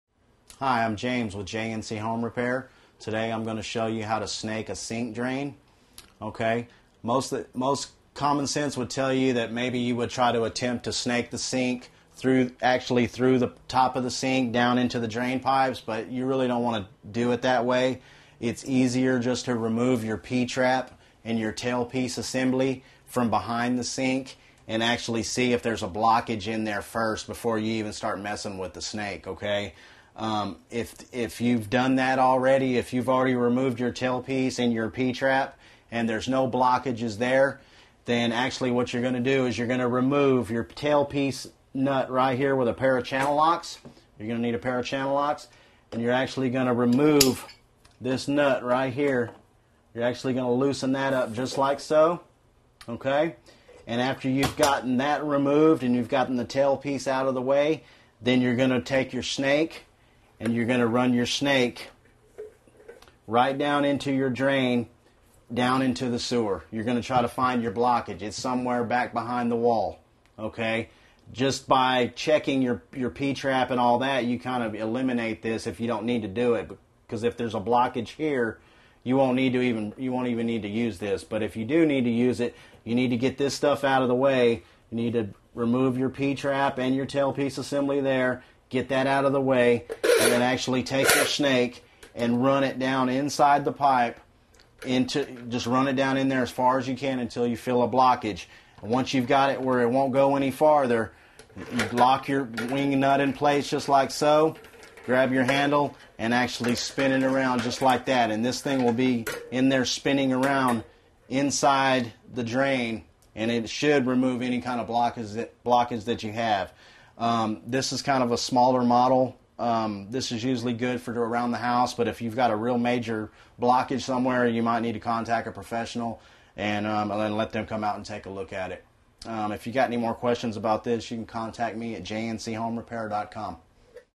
How to Snake a Sink Drain
How to Snake a Sink Drain. Part of the series: Electrical & Plumbing Repairs. Before attempting to snake a sink drain, remove the P-trap and see if there's a blockage. Snake a sink drain with tips from a certified HVAC tech and plumber in this free video on plumbing repairs.